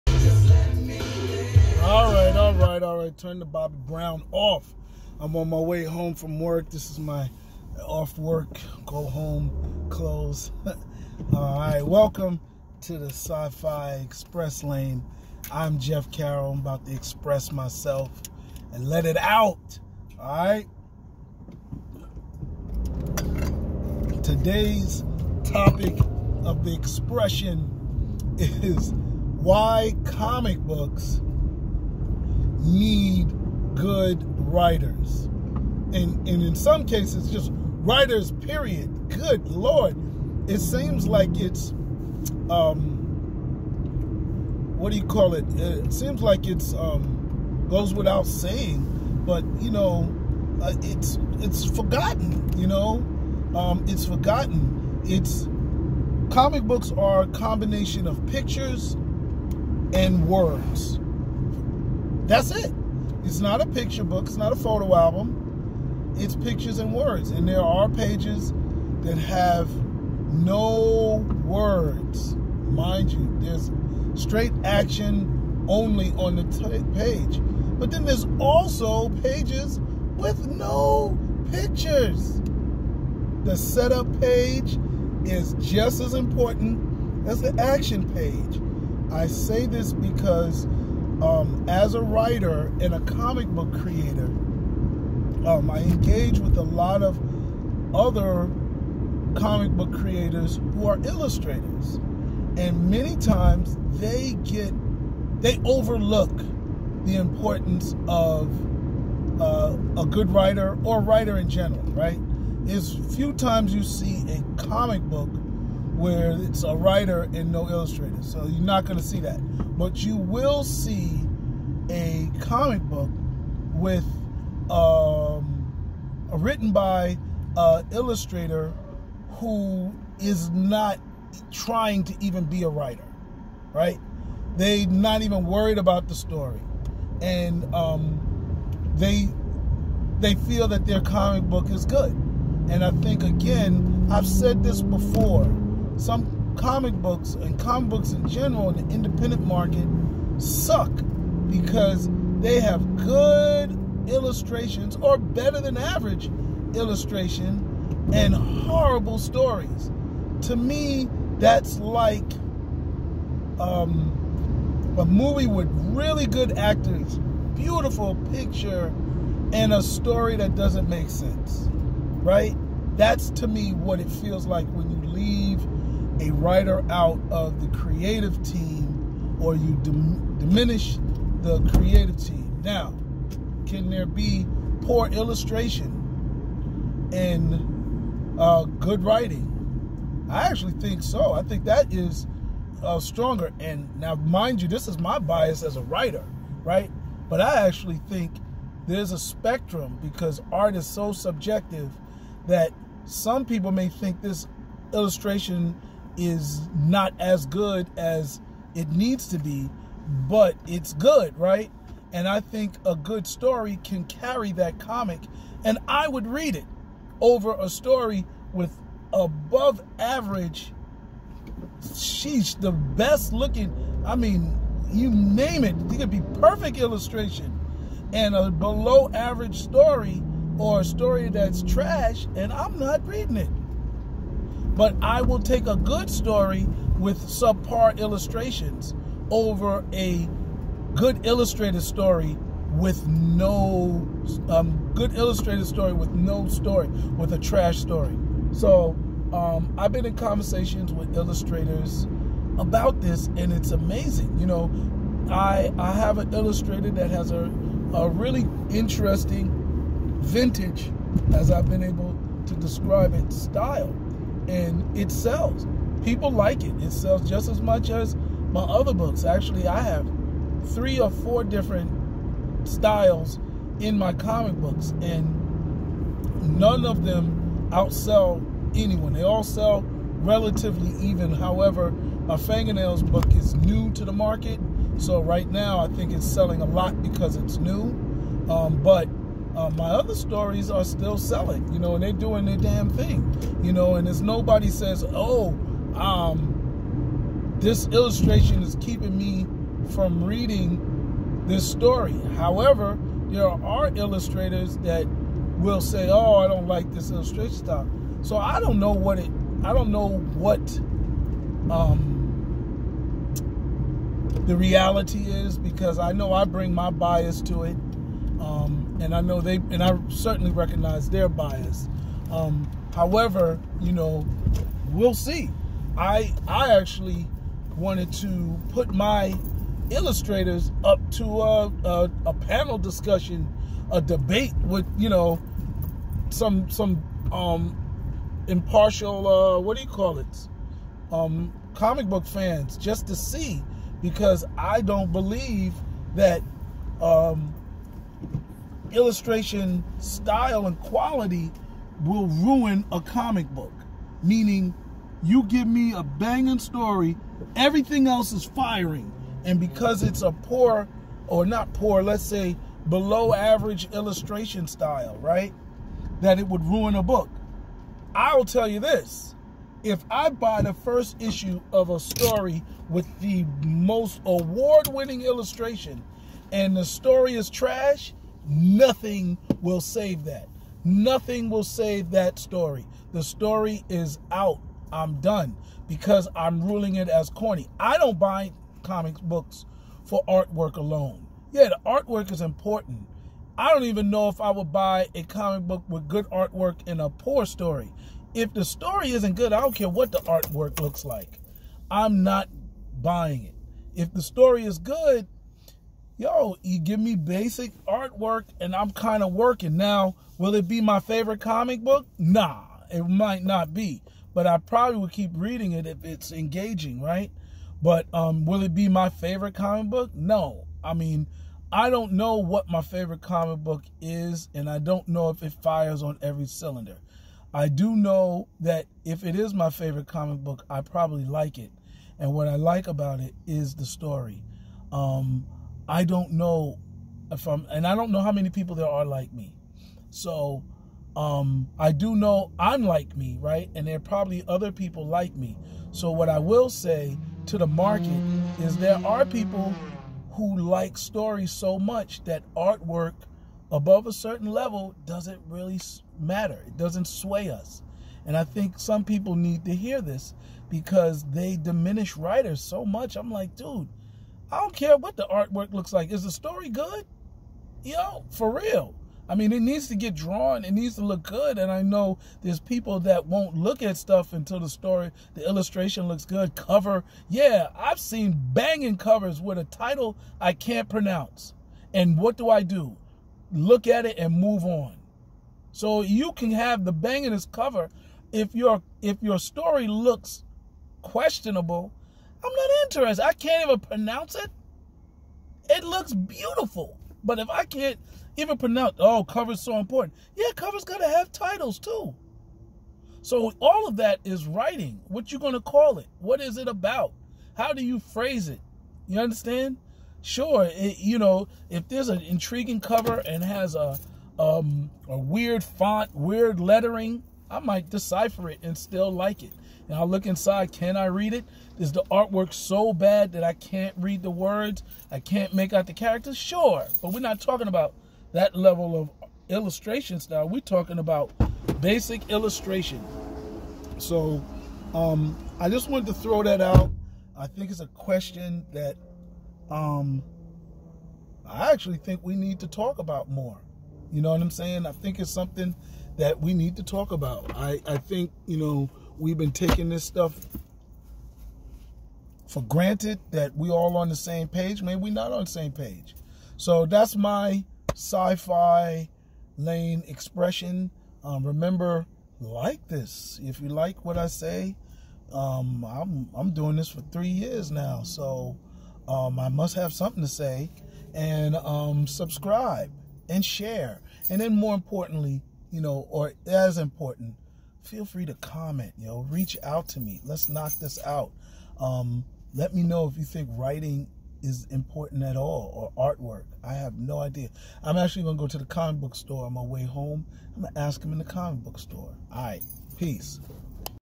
How important are writers to comic books.
Jeff Carroll Sci-fi writer and filmmaker shares his thoughts and opinions on the world of science fiction and entertainment.